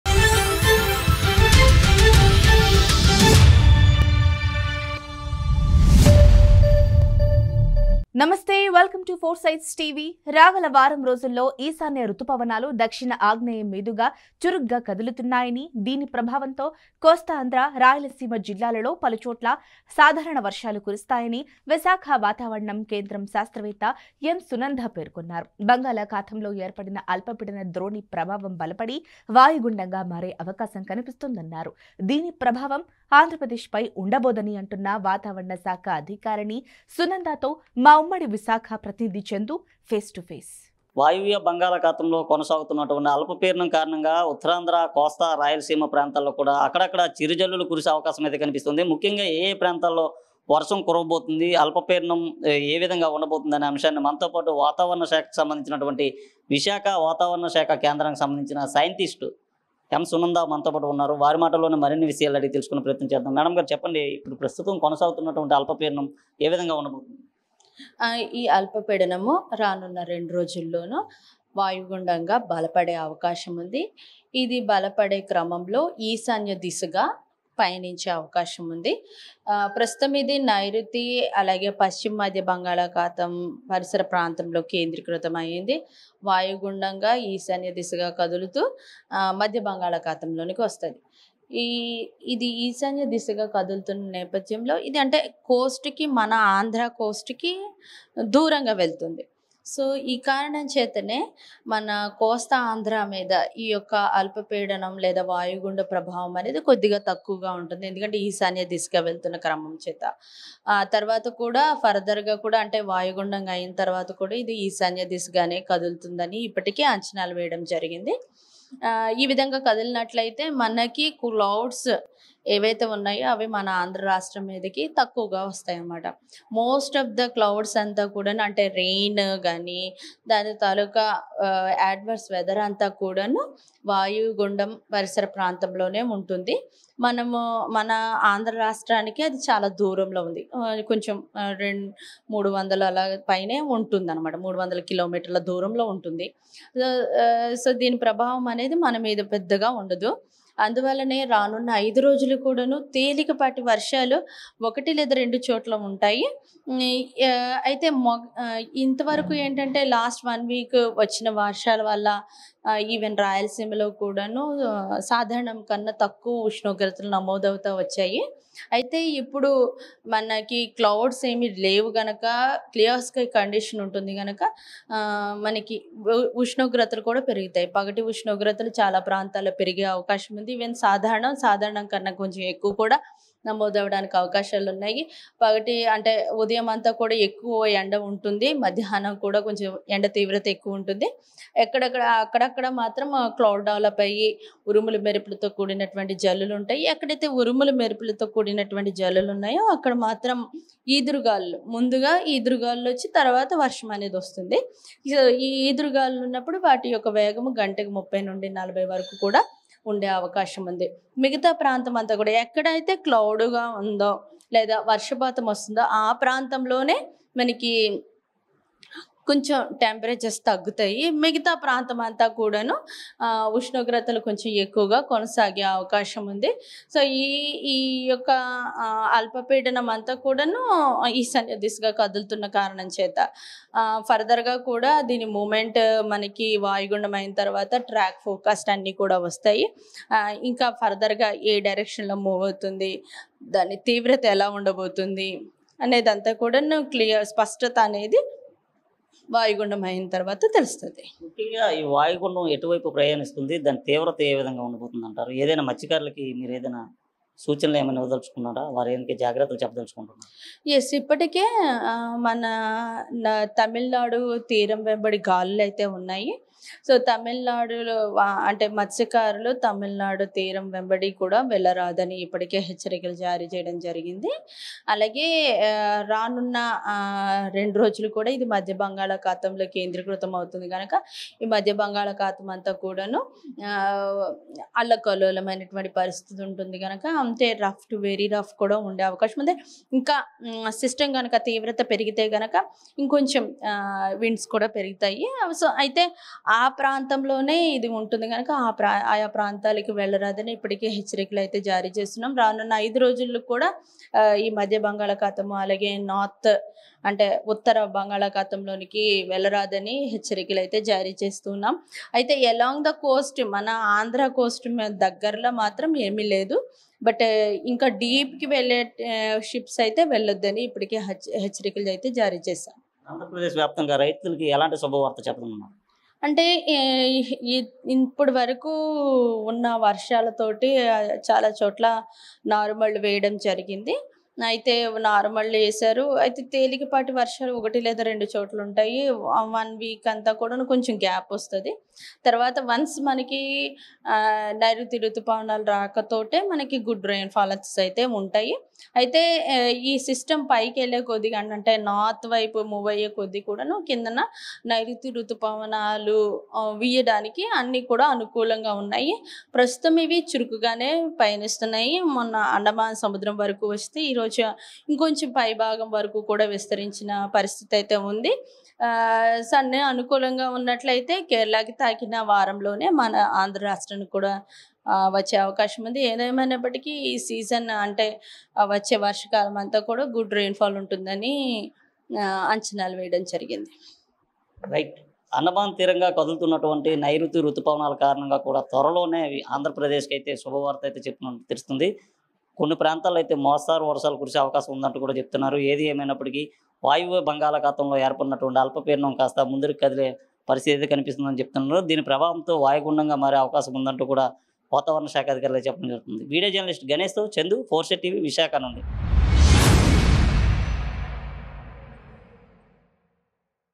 రాష్ట్ర వ్యాప్తంగా సాధారణ వర్షాలు || Visakha Meteorological Center
Visakha Meteorological Center scientist Y. Sunanda said that in the next week, the Northeast Monsoon will move actively over the South-South-East, and due to this, there will be normal rains in many places in Coast Andhra and Rayalaseema districts.